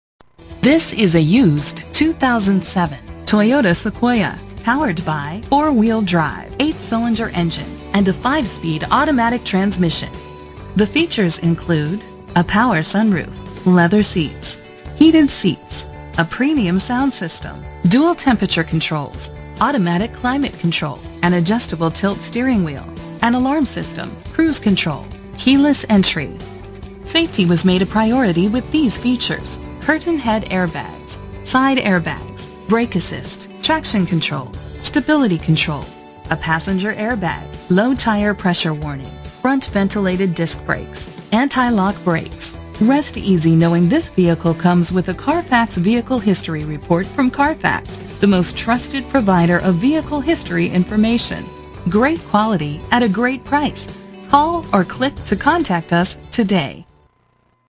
2007 Toyota Sequoia - Denver CO
Phone:
Year: 2007
Make: Toyota
Model: Sequoia
Trim: Limited
Engine: 4.7 liter 8 cylinder 32 valve MPFI DOHC
Transmission: 5-Speed Automatic
Color: Black
Mileage: 112501
Address:
2400 S. Broadway